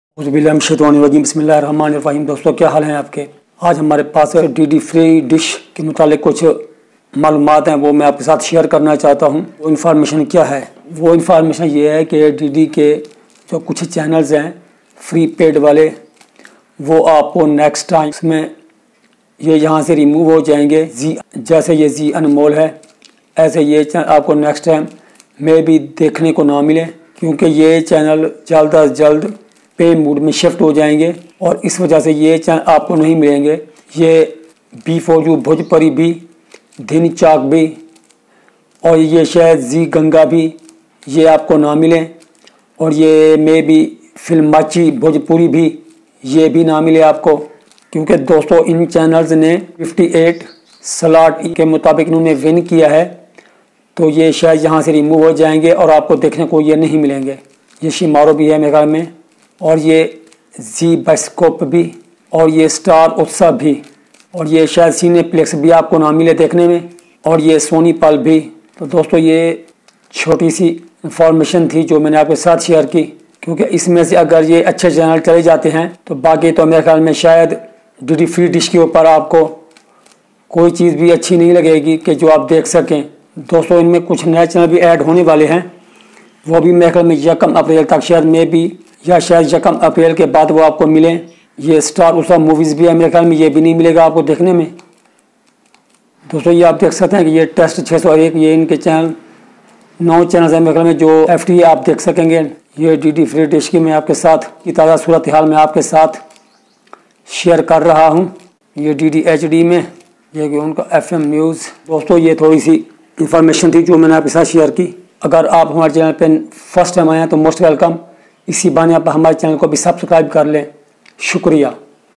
Are the DD Free Dish some FTA Channels no work on 93.5E | Dishmzg
Are the DD Free Dish some FTA Channels no work on 93.5E.
This is 1st option of the Viewers to watch FTA Channels.
DD Free Dish is a 2 Feet Dish Channels satellite.